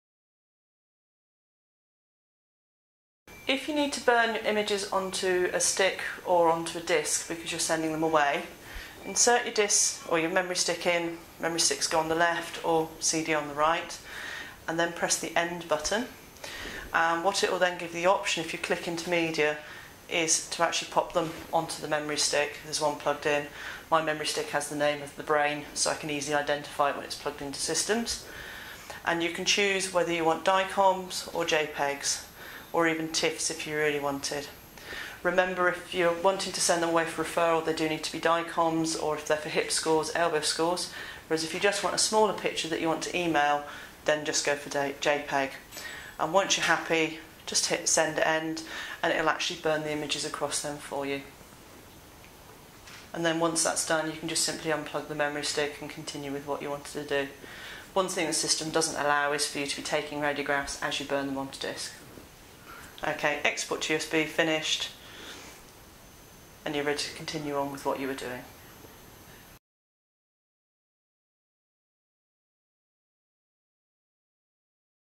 Instructional video - How to save your images to disc or USB on Cuattro Cloud Small Animal DR X-ray
BCF radiographer Kat Evans shows how to save your images to disc or USB.